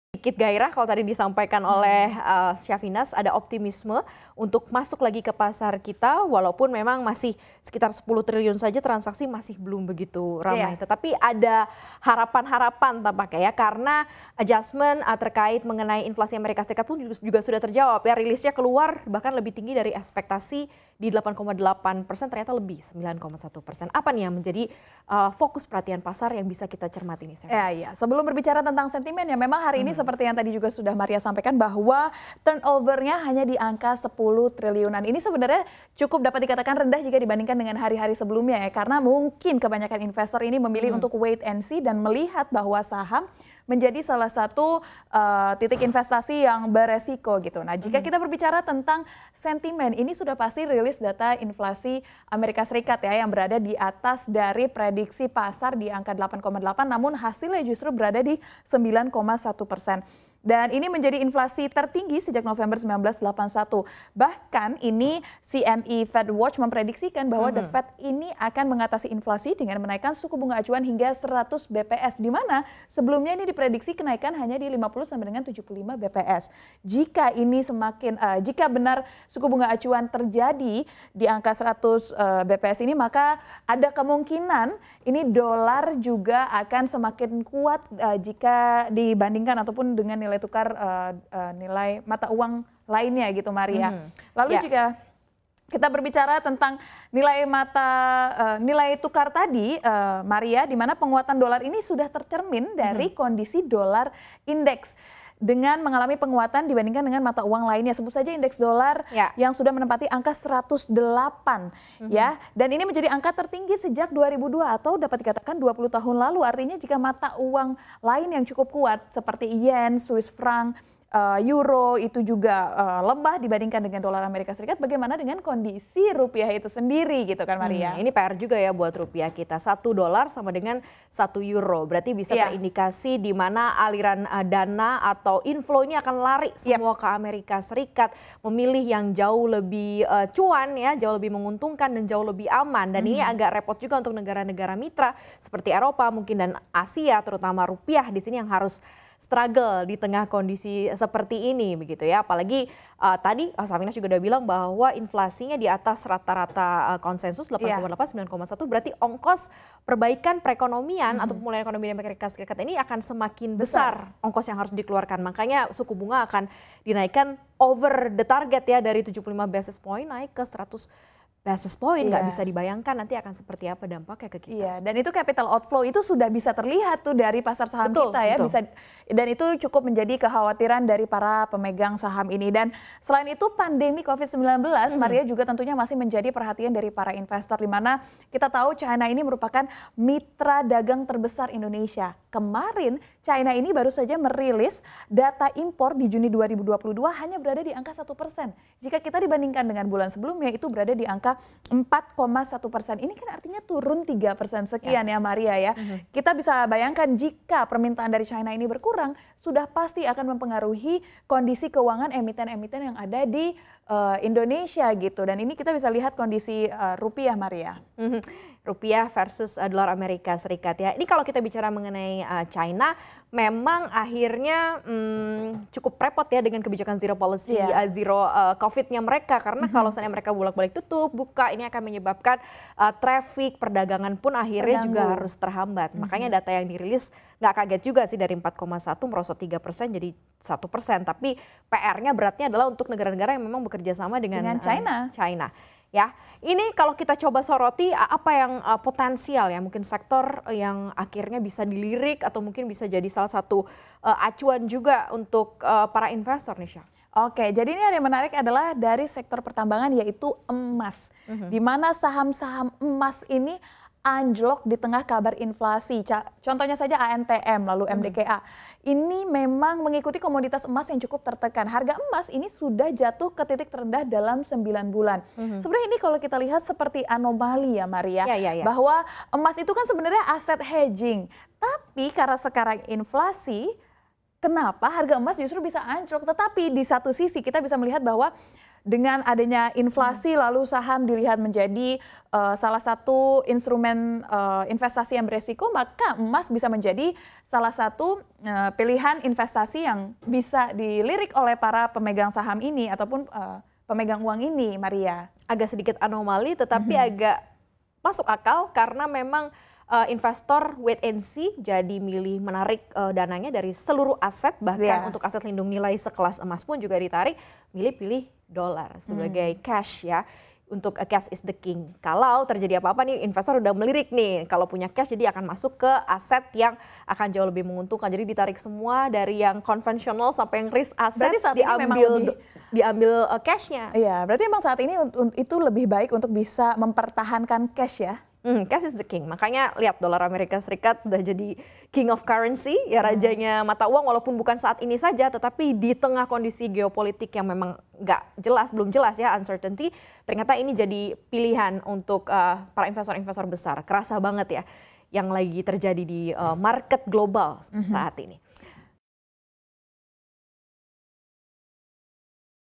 Market Focus: Inflasi Tinggi Hingga Waspada IHSG Hilang Tenaga
Berita lainnya
Tak Cuma RI, Negara G20 Ini Juga "Ogah" Kerek Suku Bunga

Indeks harga saham gabungan berhasil mempertahankan rebound dan menutup perdagangan, Kamis (14/07) di zona hijau dengan penguatan 0,74% di level 6.590.

Sentimen apa saja yang mempengaruhi pergerakan IHSG sepanjang hari ini? Selengkapnya simak ulasan Maria Katarina dan Shafinaz Nachiar dalam Closing Bell, CNBC Indonesia (Kamis, 14/07/2022)